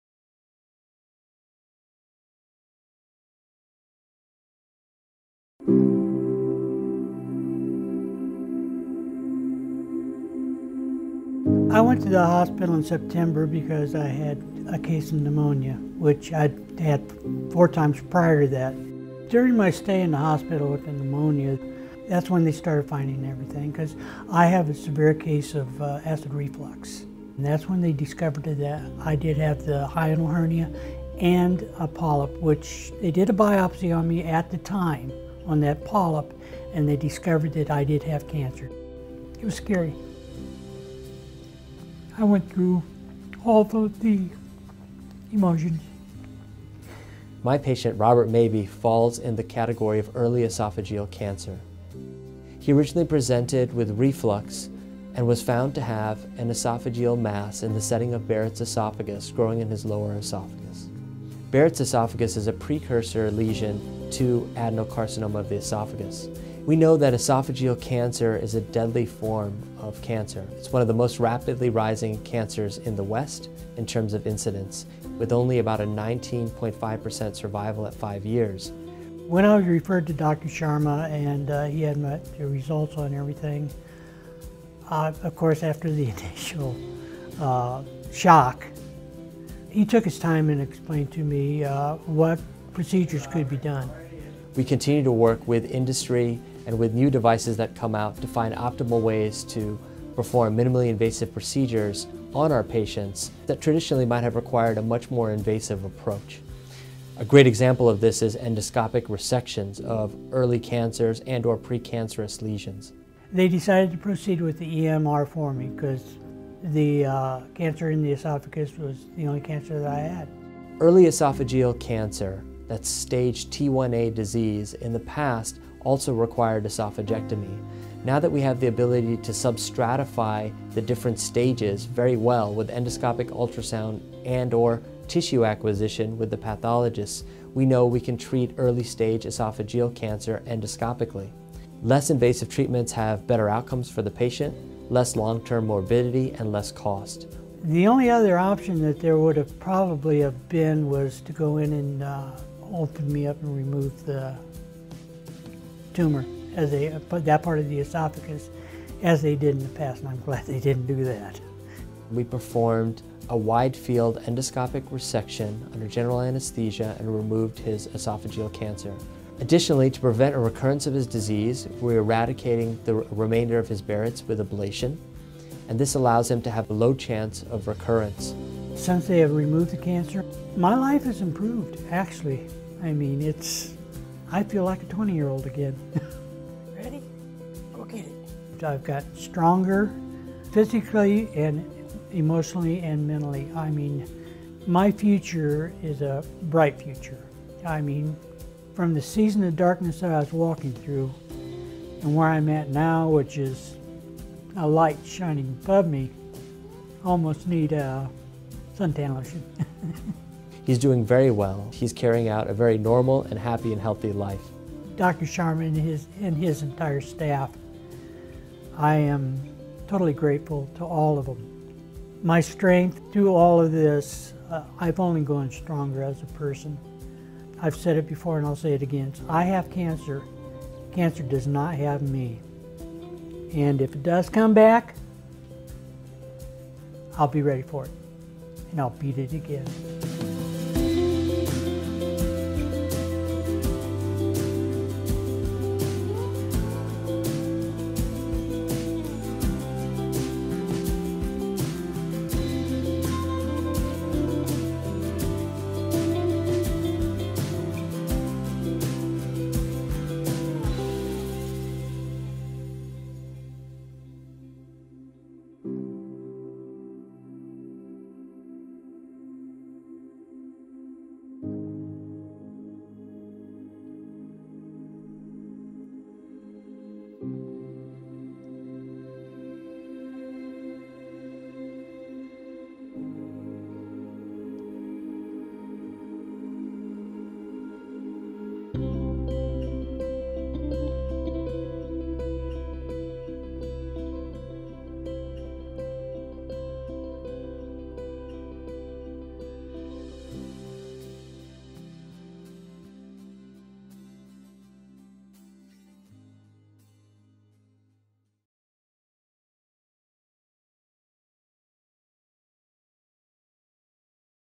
Advances in Esophageal Cancer Treatment at Parkview Cancer Institute
Neil Sharma, MD, President, Parkview Cancer Institute, PPG – Oncology, discusses advanced treatment options for Barrett’s esophagus using state of the art endoscopic membrane resection (EMR) technology.